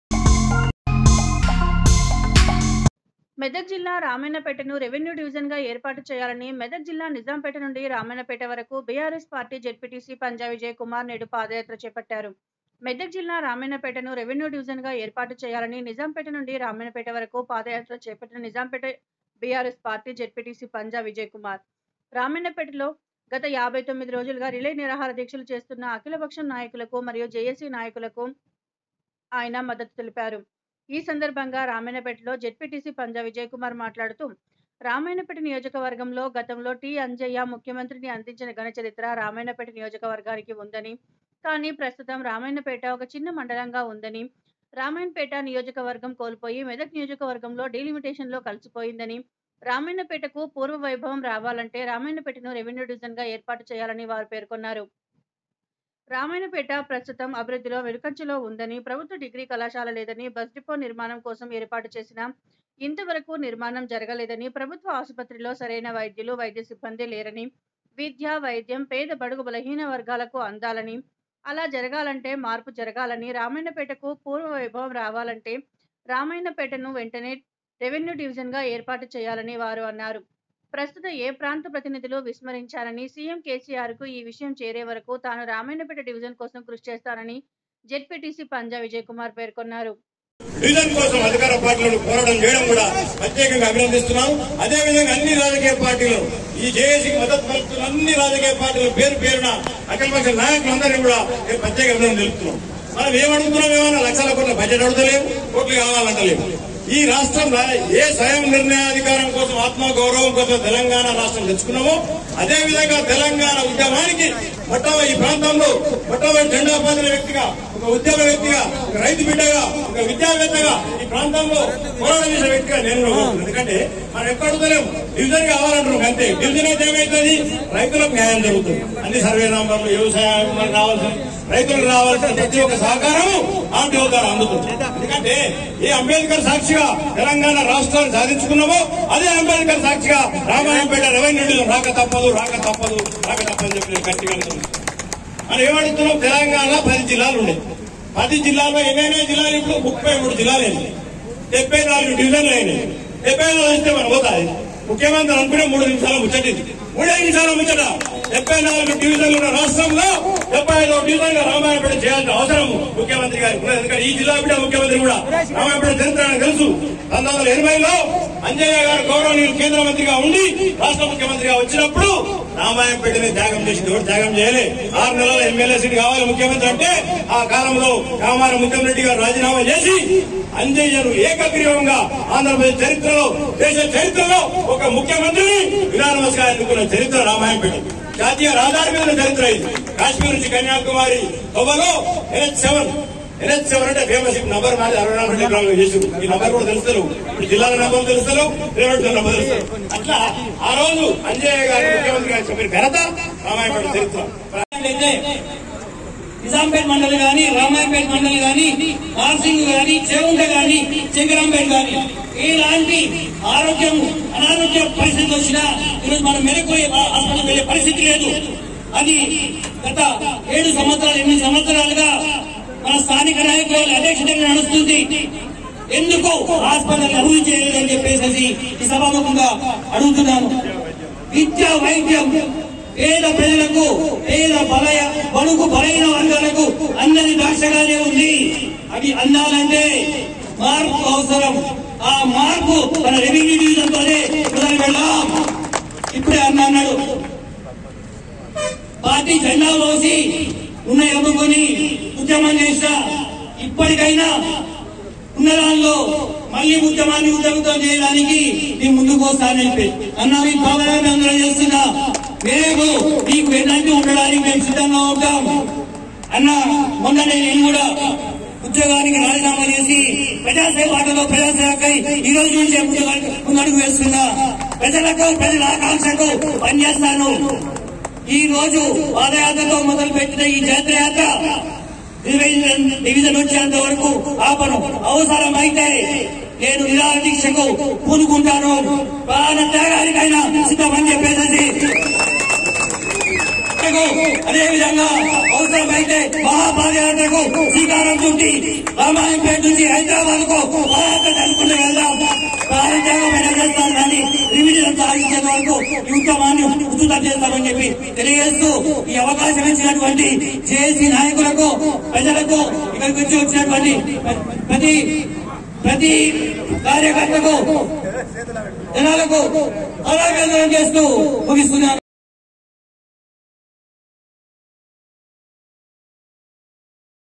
రామాయంపేటను రెవెన్యూ డివిజన్ గా చేయాలని మెదక్ జిల్లా నిజాంపేట నుండి రామయంపేట పాదయాత్ర
మెదక్ జిల్లా రామాయంపేటను రెవెన్యూ డివిజన్ గా ఏర్పాటు చేయాలని మెదక్ జిల్లా నిజాంపేట నుండి రామయంపేట వరకు బిఆర్ఎస్ పార్టీ జడ్పిటిసి పంజా విజయ్ కుమార్ నేడు పాదయాత్ర